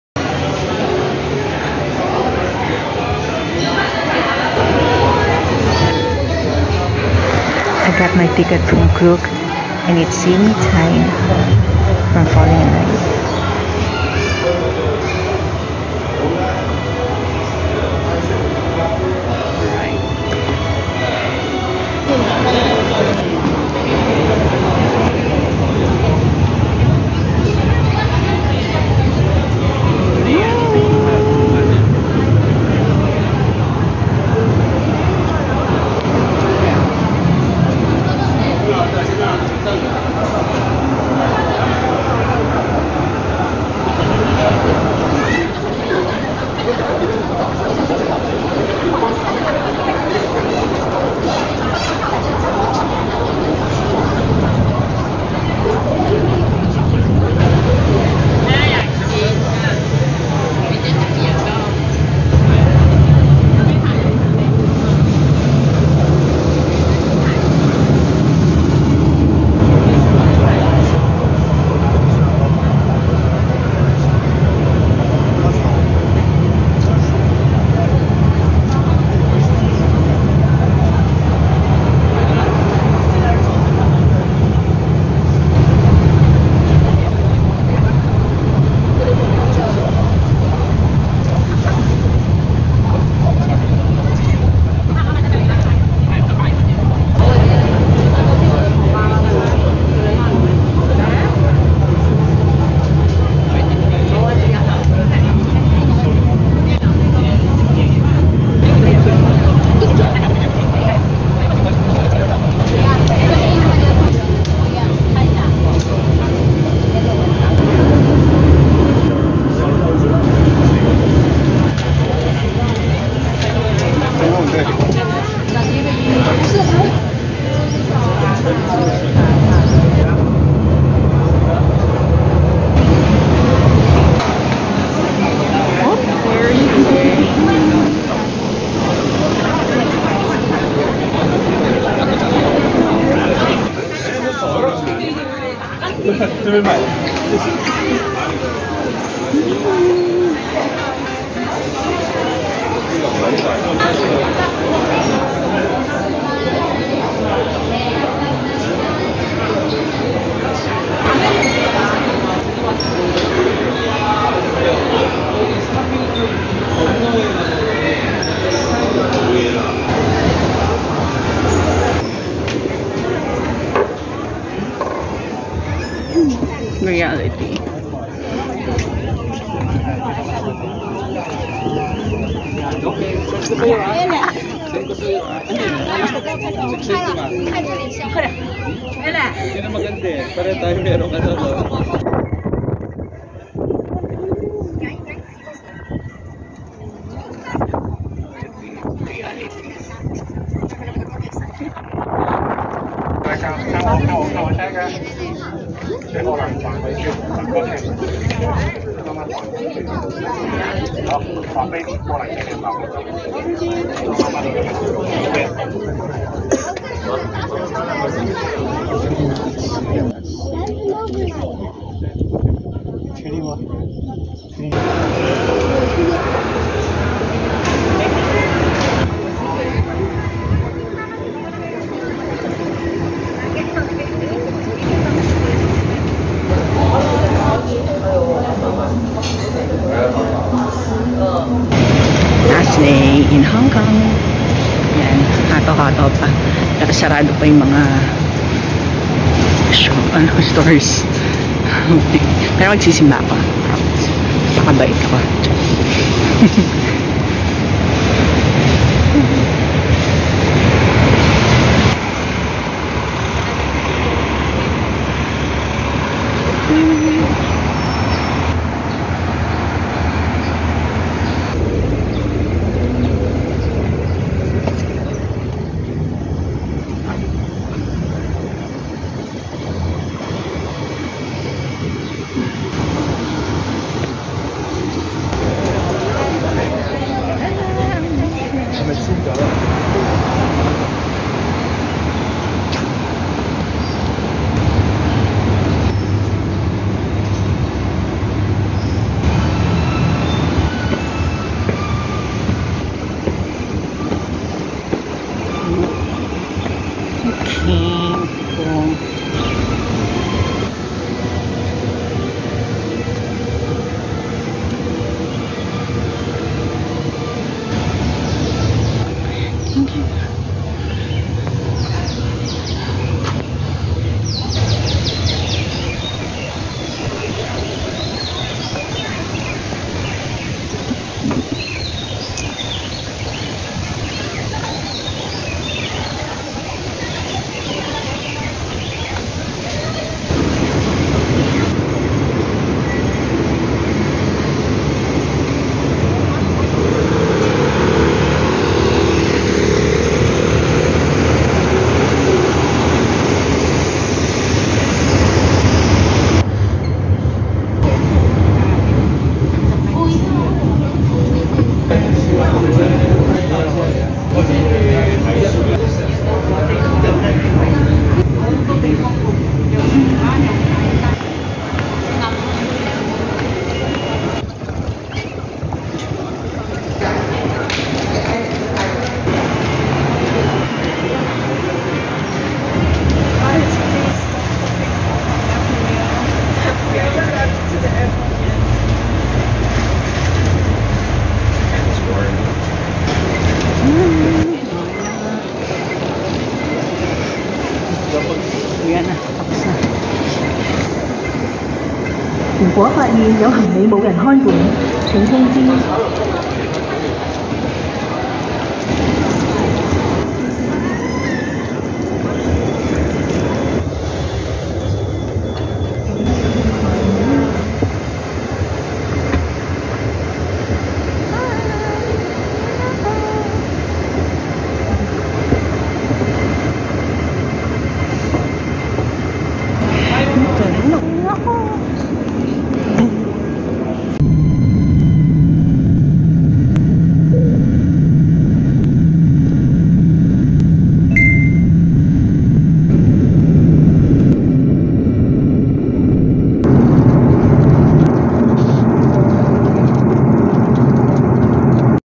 Hong Kong 2023 - The Peak and my last day.
Second part of my Day 2 is going up the Peak and my last day in Hong Kong.